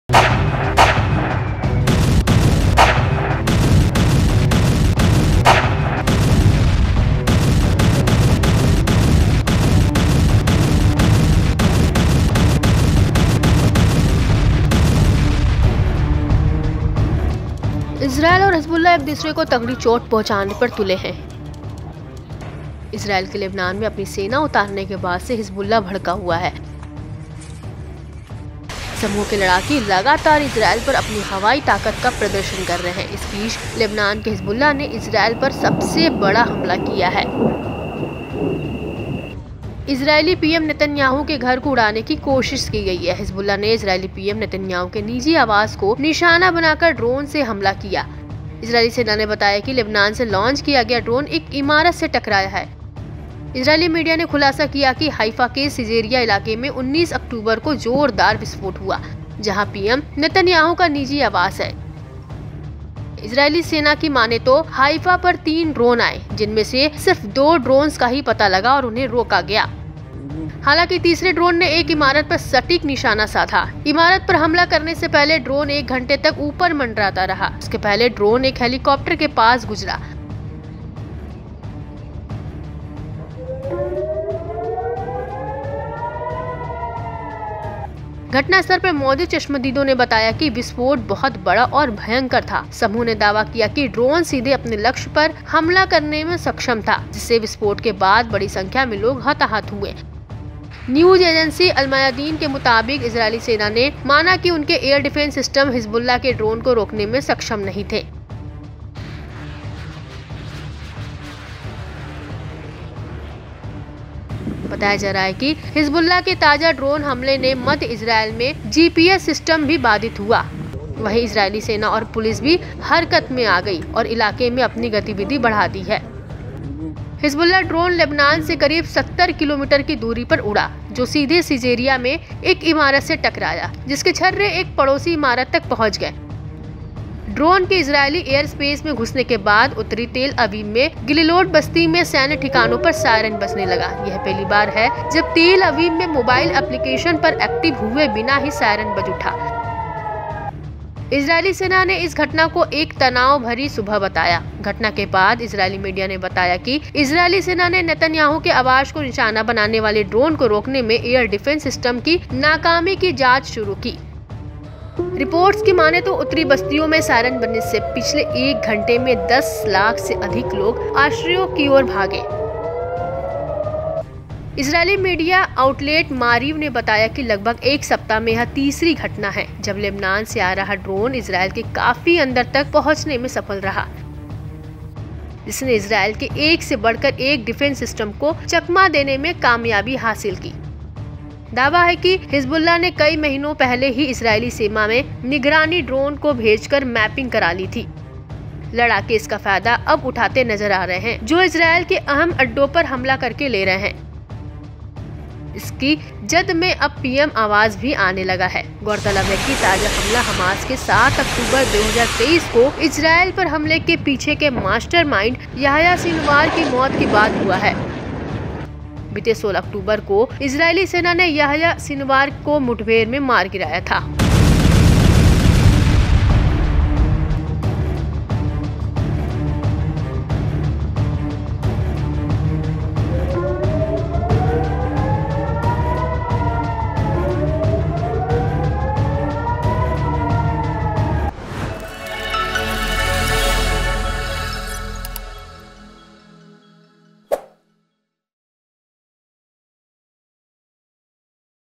Israel Hezbollah War: हिज़बुल्लाह के अटैक से खौफ़ में नेतन्याहू ! Drone Attack| PM Residence |N18G
Israel and Hezbollah are hell-bent on inflicting heavy damage on each other. Hezbollah is furious since Israel landed its army in Lebanon. The group's fighters are constantly demonstrating their air power on Israel. Meanwhile, Lebanon's Hezbollah has carried out the biggest attack on Israel. An attempt has been made to blow up Israeli PM Benjamin Netanyahu's house. Hezbollah carried out a drone attack targeting Israeli PM Netanyahu's personal residence. The Israeli army said that the drone launched from Lebanon has hit a building. Israeli media revealed that a massive explosion took place on October 19 in the Caesarea area of ​​Haifa. Where PM Netanyahu's personal residence is located. According to the Israeli army, three drones came over Haifa, out of which only two drones were detected and stopped. However, the third drone accurately targeted a building. The drone hovered over the building for an hour before attacking it. Before that, the drone passed near a helicopter. Eyewitnesses present at the spot said that the explosion was very big and terrible.

इजरायल और हिजबुल्लाह एक दूसरे को तगड़ी चोट पहुंचाने पर तूले हैं. इजरायल के लेबनान में अपनी सेना उतारने के बाद से हिजबुल्लाह भड़का हुआ है. समूह के लड़ाके लगातार इजरायल पर अपनी हवाई ताकत का प्रदर्शन कर रहे हैं. इस बीच, लेबनान के हिजबुल्लाह ने इजरायल पर सबसे बड़ा हमला किया है. इजरायली पीएम बेंजामिन नेतन्याहू के घर को उड़ाने की कोशिश की गई है. हिजबुल्लाह ने इजरायली पीएम नेतन्याहू के निजी आवास को निशाना बनाकर ड्रोन हमला किया. इजरायली सेना ने बताया कि लेबनान से लॉन्च किया गया ड्रोन एक इमारत से टकराया है. इजरायली मीडिया ने खुलासा किया कि हाइफा के सीजेरिया इलाके में 19 अक्टूबर को जोरदार विस्फोट हुआ. जहां पीएम नेतन्याहू का निजी आवास है. इजरायली सेना की माने तो हाइफ़ा पर तीन ड्रोन आए, जिनमें से सिर्फ दो ड्रोन्स का ही पता लगा और उन्हें रोका गया. हालांकि, तीसरे ड्रोन ने एक इमारत पर सटीक निशाना साधा. इमारत पर हमला करने से पहले ड्रोन एक घंटे तक ऊपर मंडराता रहा. उसके पहले ड्रोन एक हेलीकॉप्टर के पास गुजरा. घटनास्थल पर मौजूद चश्मदीदों ने बताया कि विस्फोट बहुत बड़ा और भयंकर था.